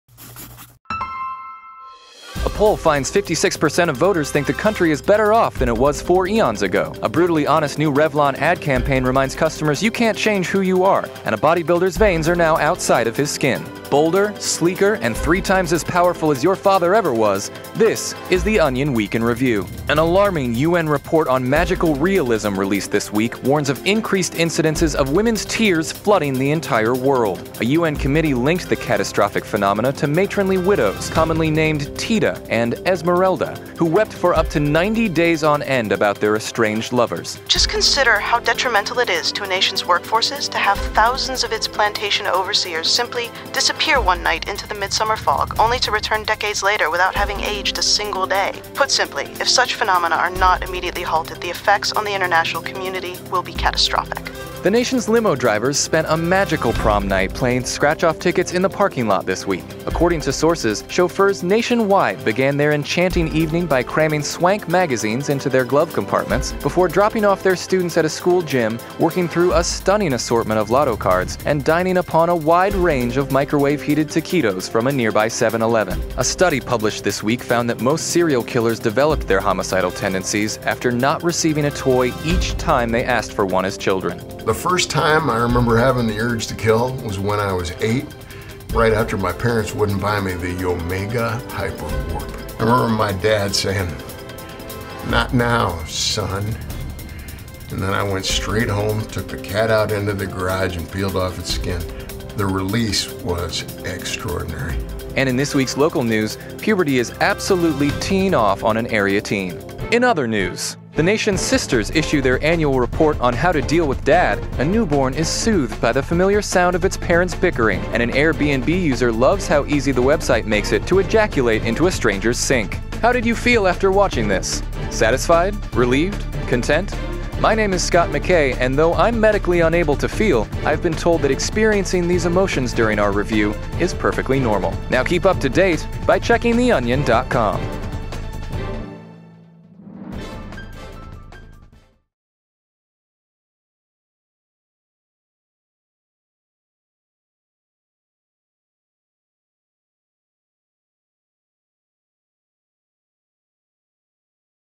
The Onion Review -- Week Of May 9th, 2014
A poll finds 56% of voters think the country is better off than it was 4 eons ago, a brutally honest new Revlon ad campaign reminds customers you can't change who you are, and a bodybuilder's veins are now outside of his skin. It's the week of May 9, 2014.